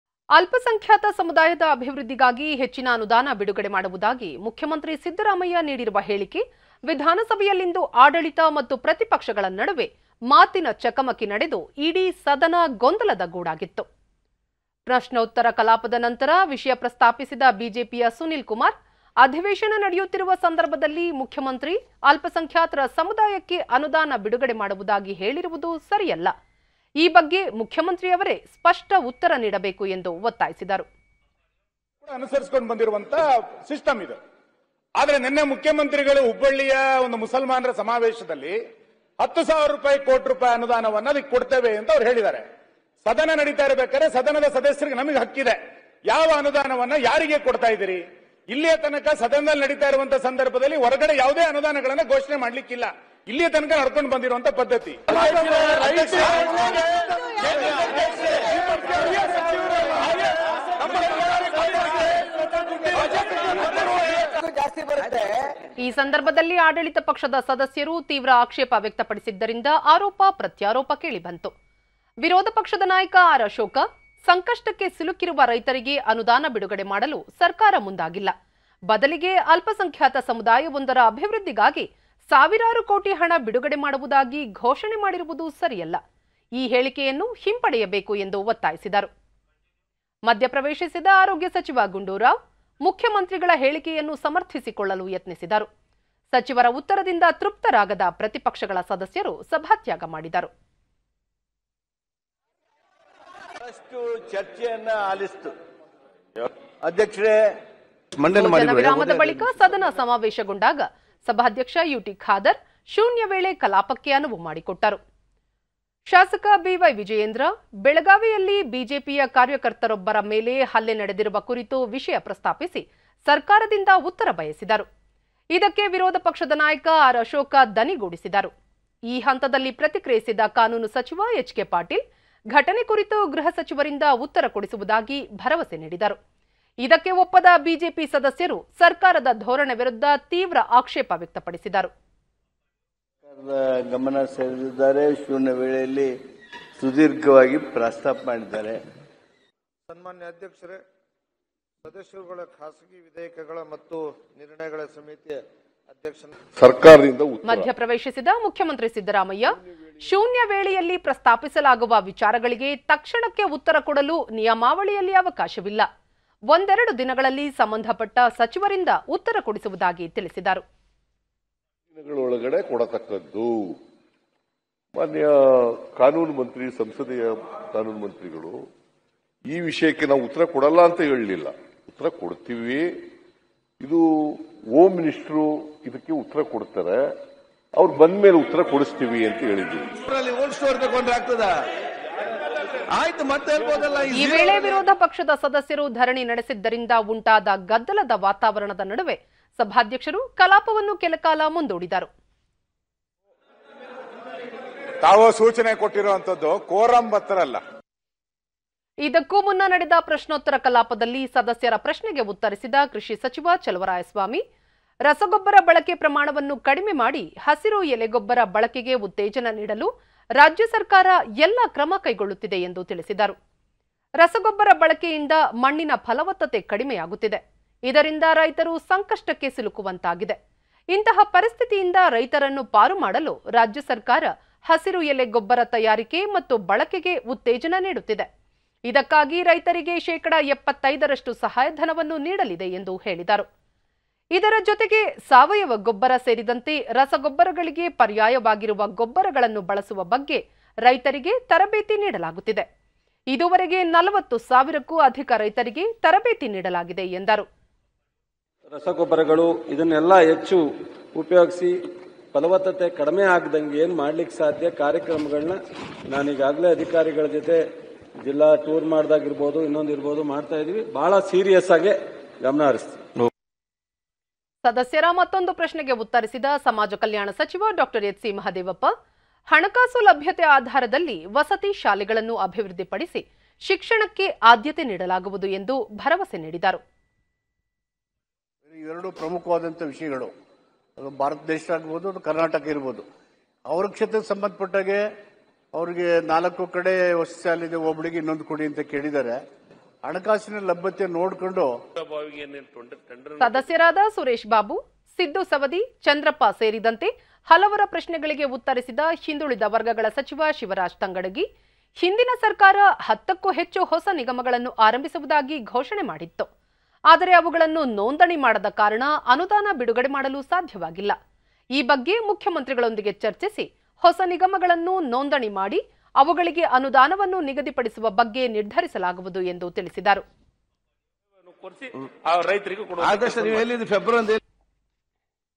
ಹಸಿರು ಎಲೆ ಗೊಬ್ಬರ ಬಳಕೆಗೆ ಉತ್ತೇಜನ; ಕೃಷಿ ಸಚಿವ ಚಲುವರಾಯಸ್ವಾಮಿ ಮಾಹಿತಿ
ಅಲ್ಪ ಸಂಖ್ಯಾತ ಸಮುದಾಯದ ಅಭಿವೃದ್ಧಿಗಾಗಿ ಹೆಚ್ಚಿನ ಅನುದಾನ ಬಿಡುಗಡೆ ಮಾಡುವುದಾಗಿ ಮುಖ್ಯಮಂತ್ರಿ ಸಿದ್ದರಾಮಯ್ಯ ನೀಡಿರುವ ಹೇಳಿಕೆ ವಿಧಾನಸಭೆಯಲ್ಲಿಂದು ಆಡಳಿತ ಮತ್ತು ಪ್ರತಿಪಕ್ಷಗಳ ನಡುವೆ ಮಾತಿನ ಚಕಮಕಿ ನಡೆದು ಇಡೀ ಸದನ ಗೊಂದಲದ ಗೂಡಾಗಿತ್ತು.
ಪ್ರಶ್ನೋತ್ತರ ಕಲಾಪದ ನಂತರ ವಿಷಯ ಪ್ರಸ್ತಾಪಿಸಿದ ಬಿಜೆಪಿಯ ಸುನೀಲ್ ಕುಮಾರ್, ಅಧಿವೇಶನ ನಡೆಯುತ್ತಿರುವ ಸಂದರ್ಭದಲ್ಲಿ ಮುಖ್ಯಮಂತ್ರಿ ಅಲ್ಪಸಂಖ್ಯಾತರ ಸಮುದಾಯಕ್ಕೆ ಅನುದಾನ ಬಿಡುಗಡೆ ಮಾಡುವುದಾಗಿ ಹೇಳಿರುವುದು ಸರಿಯಲ್ಲ. ಈ ಬಗ್ಗೆ ಮುಖ್ಯಮಂತ್ರಿಯವರೇ ಸ್ಪಷ್ಟ ಉತ್ತರ ನೀಡಬೇಕು ಎಂದು ಒತ್ತಾಯಿಸಿದರು.
ಈ ಸಂದರ್ಭದಲ್ಲಿ ಆಡಳಿತ ಪಕ್ಷದ ಸದಸ್ಯರು ತೀವ್ರ ಆಕ್ಷೇಪ ವ್ಯಕ್ತಪಡಿಸಿದ್ದರಿಂದ ಆರೋಪ-ಪ್ರತ್ಯಾರೋಪ ಕೇಳಿಬಂತು.
ವಿರೋಧ ಪಕ್ಷದ ನಾಯಕ ಆರ್. ಅಶೋಕ, ಸಂಕಷ್ಟಕ್ಕೆ ಸಿಲುಕಿರುವ ರೈತರಿಗೆ ಅನುದಾನ ಬಿಡುಗಡೆ ಮಾಡಲು ಸರ್ಕಾರ ಮುಂದಾಗಿಲ್ಲ. ಬದಲಿಗೆ ಅಲ್ಪಸಂಖ್ಯಾತ ಸಮುದಾಯದವೊಂದರ ಅಭಿವೃದ್ಧಿಗಾಗಿ ಸಾವಿರಾರು ಕೋಟಿ ಹಣ ಬಿಡುಗಡೆ ಮಾಡುವುದಾಗಿ ಘೋಷಣೆ ಮಾಡಿರುವುದು ಸರಿಯಲ್ಲ. ಈ ಹೇಳಿಕೆಯನ್ನು ಹಿಂಪಡೆಯಬೇಕು ಎಂದು ಒತ್ತಾಯಿಸಿದರು.
ಮಧ್ಯ ಪ್ರವೇಶಿಸಿದ ಆರೋಗ್ಯ ಸಚಿವ ಗುಂಡೂರಾವ್, ಮುಖ್ಯಮಂತ್ರಿಗಳ  ಹೇಳಿಕೆಯನ್ನು ಸಮರ್ಥಿಸಿಕೊಳ್ಳಲು ಯತ್ನಿಸಿದರು.
ಸಚಿವರ ಉತ್ತರದಿಂದ ತೃಪ್ತರಾಗದ ಪ್ರತಿಪಕ್ಷಗಳ ಸದಸ್ಯರು ಸಭಾತ್ಯಾಗ ಮಾಡಿದರು.
 ಭೋಜನ ವಿರಾಮದ ಬಳಿಕ ಸದನ ಸಮಾವೇಶಗೊಂಡಾಗ ಸಭಾಧ್ಯಕ್ಷ ಯು.ಟಿ. ಖಾದರ್ ಶೂನ್ಯವೇಳೆ ಕಲಾಪಕ್ಕೆ ಅನುವು ಮಾಡಿಕೊಟ್ಟರು.
 ಶಾಸಕ ಬಿ.ವೈ. ವಿಜಯೇಂದ್ರ, ಬೆಳಗಾವಿಯಲ್ಲಿ ಬಿಜೆಪಿಯ ಕಾರ್ಯಕರ್ತರೊಬ್ಬರ ಮೇಲೆ ಹಲ್ಲೆ ನಡೆದಿರುವ ಕುರಿತು ವಿಷಯ ಪ್ರಸ್ತಾಪಿಸಿ ಸರ್ಕಾರದಿಂದ ಉತ್ತರ ಬಯಸಿದರು.  ಇದಕ್ಕೆ  ವಿರೋಧ ಪಕ್ಷದ ನಾಯಕ ಆರ್. ಅಶೋಕ ದನಿಗೂಡಿಸಿದರು.  ಈ ಹಂತದಲ್ಲಿ ಪ್ರತಿಕ್ರಿಯಿಸಿದ ಕಾನೂನು ಸಚಿವ ಎಚ್.ಕೆ. ಪಾಟೀಲ್, ಘಟನೆ ಕುರಿತು ಗೃಹ ಸಚಿವರಿಂದ ಉತ್ತರ ಕೊಡಿಸುವುದಾಗಿ ಭರವಸೆ ನೀಡಿದರು.
 ಇದಕ್ಕೆ ಒಪ್ಪದ ಬಿಜೆಪಿ ಸದಸ್ಯರು ಸರ್ಕಾರದ ಧೋರಣೆ ವಿರುದ್ಧ ತೀವ್ರ ಆಕ್ಷೇಪ ವ್ಯಕ್ತಪಡಿಸಿದರು.
ಮಧ್ಯಪ್ರವೇಶಿಸಿದ ಮುಖ್ಯಮಂತ್ರಿ ಸಿದ್ದರಾಮಯ್ಯ, ಶೂನ್ಯ ವೇಳೆಯಲ್ಲಿ ಪ್ರಸ್ತಾಪಿಸಲಾಗುವ ವಿಚಾರಗಳಿಗೆ ತಕ್ಷಣಕ್ಕೆ ಉತ್ತರ ಕೊಡಲು ನಿಯಮಾವಳಿಯಲ್ಲಿ ಅವಕಾಶವಿಲ್ಲ. ಒಂದೆರಡು ದಿನಗಳಲ್ಲಿ ಸಂಬಂಧಪಟ್ಟ ಸಚಿವರಿಂದ ಉತ್ತರ ಕೊಡಿಸುವುದಾಗಿ ತಿಳಿಸಿದರು.
ಈ ವೇಳೆ ವಿರೋಧ ಪಕ್ಷದ ಸದಸ್ಯರು ಧರಣಿ ನಡೆಸಿದ್ದರಿಂದ ಉಂಟಾದ ಗದ್ದಲದ ವಾತಾವರಣದ ನಡುವೆ ಸಭಾಧ್ಯಕ್ಷರು ಕಲಾಪವನ್ನು ಕೆಲಕಾಲ ಮುಂದೂಡಿದರು.
ಇದಕ್ಕೂ ಮುನ್ನ ನಡೆದ ಪ್ರಶ್ನೋತ್ತರ ಕಲಾಪದಲ್ಲಿ ಸದಸ್ಯರ ಪ್ರಶ್ನೆಗೆ ಉತ್ತರಿಸಿದ ಕೃಷಿ ಸಚಿವ ಚಲುವರಾಯಸ್ವಾಮಿ, ರಸಗೊಬ್ಬರ  ಬಳಕೆ ಪ್ರಮಾಣವನ್ನು ಕಡಿಮೆ ಮಾಡಿ,  ಹಸಿರು ಎಲೆ ಗೊಬ್ಬರ  ಬಳಕೆಗೆ ಉತ್ತೇಜನ ನೀಡಲು  ರಾಜ್ಯ ಸರ್ಕಾರ ಎಲ್ಲ ಕ್ರಮ ಕೈಗೊಳ್ಳುತ್ತಿದೆ ಎಂದು ತಿಳಿಸಿದರು.
ರಸಗೊಬ್ಬರ ಬಳಕೆಯಿಂದ ಮಣ್ಣಿನ ಫಲವತ್ತತೆ ಕಡಿಮೆಯಾಗುತ್ತಿದೆ. ಇದರಿಂದ ರೈತರು  ಸಂಕಷ್ಟಕ್ಕೆ ಸಿಲುಕುವಂತಾಗಿದೆ.  ಇಂತಹ ಪರಿಸ್ಥಿತಿಯಿಂದ ರೈತರನ್ನು ಪಾರು ಮಾಡಲು  ರಾಜ್ಯ ಸರ್ಕಾರ ಹಸಿರು ಎಲೆ ಗೊಬ್ಬರ  ತಯಾರಿಕೆ ಮತ್ತು ಬಳಕೆಗೆ  ಉತ್ತೇಜನ ನೀಡುತ್ತಿದೆ . ಇದಕ್ಕಾಗಿ ರೈತರಿಗೆ  ಶೇಕಡ  75ರಷ್ಟು  ಸಹಾಯಧನವನ್ನು ನೀಡಲಿದೆ ಎಂದು ಹೇಳಿದರು.
ಇದರ ಜೊತೆಗೆ ಸಾವಯವ ಗೊಬ್ಬರ ಸೇರಿದಂತೆ  ರಸಗೊಬ್ಬರಗಳಿಗೆ ಪರ್ಯಾಯವಾಗಿರುವ ಗೊಬ್ಬರಗಳನ್ನು ಬಳಸುವ  ಬಗ್ಗೆ ರೈತರಿಗೆ  ತರಬೇತಿ ನೀಡಲಾಗುತ್ತಿದೆ. ಇದುವರೆಗೆ 40 ಸಾವಿರಕ್ಕೂ ಅಧಿಕ ರೈತರಿಗೆ ತರಬೇತಿ ನೀಡಲಾಗಿದೆ ಎಂದರು.

ಸದಸ್ಯರ ಮತ್ತೊಂದು ಪ್ರಶ್ನೆಗೆ ಉತ್ತರಿಸಿದ ಸಮಾಜ ಕಲ್ಯಾಣ ಸಚಿವ ಡಾ. ಎಚ್. ಸಿ. ಮಹದೇವಪ್ಪ,  ಹಣಕಾಸು ಲಭ್ಯತೆ ಆಧಾರದಲ್ಲಿ ವಸತಿ ಶಾಲೆಗಳನ್ನು ಅಭಿವೃದ್ಧಿಪಡಿಸಿ, ಶಿಕ್ಷಣಕ್ಕೆ ಆದ್ಯತೆ ನೀಡಲಾಗುವುದು ಎಂದು ಭರವಸೆ ನೀಡಿದರು.
ಸದಸ್ಯರಾದ  ಸುರೇಶ್ ಬಾಬು, ಸಿದ್ದು ಸವದಿ, ಚಂದ್ರಪ್ಪ ಸೇರಿದಂತೆ  ಹಲವರ ಪ್ರಶ್ನೆಗಳಿಗೆ ಉತ್ತರಿಸಿದ  ಹಿಂದುಳಿದ ವರ್ಗಗಳ  ಸಚಿವ  ಶಿವರಾಜ್ ತಂಗಡಗಿ, ಹಿಂದಿನ ಸರ್ಕಾರ  ಹತ್ತಕ್ಕೂ ಹೆಚ್ಚು  ಹೊಸ ನಿಗಮಗಳನ್ನು  ಆರಂಭಿಸುವುದಾಗಿ ಘೋಷಣೆ ಮಾಡಿತ್ತು. ಆದರೆ, ಅವುಗಳನ್ನು ನೋಂದಣಿ ಮಾಡದ ಕಾರಣ  ಅನುದಾನ ಬಿಡುಗಡೆ ಮಾಡಲು ಸಾಧ್ಯವಾಗಿಲ್ಲ.  ಈ ಬಗ್ಗೆ ಮುಖ್ಯಮಂತ್ರಿಗಳೊಂದಿಗೆ ಚರ್ಚಿಸಿ, ಹೊಸ ನಿಗಮಗಳನ್ನು ನೋಂದಣಿ ಮಾಡಿ, ಅವುಗಳಿಗೆ ಅನುದಾನವನ್ನು ನಿಗದಿಪಡಿಸುವ ಬಗ್ಗೆ ನಿರ್ಧರಿಸಲಾಗುವುದು ಎಂದು ತಿಳಿಸಿದರು.